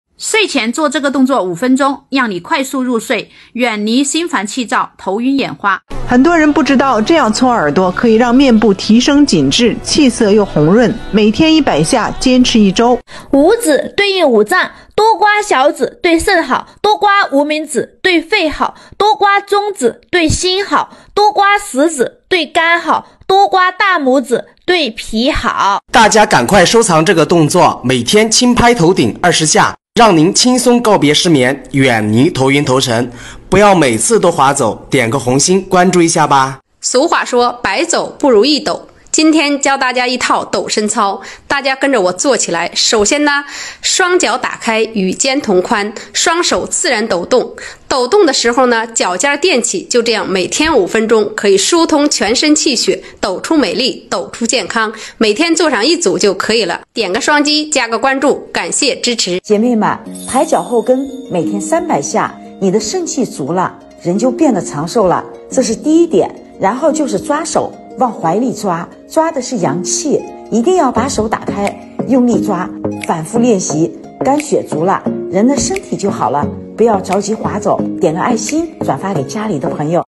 保健動作_供大家參考！
影片裡的動作，不用花錢買器材！加減試看看…也許對病痛抒緩有幫助。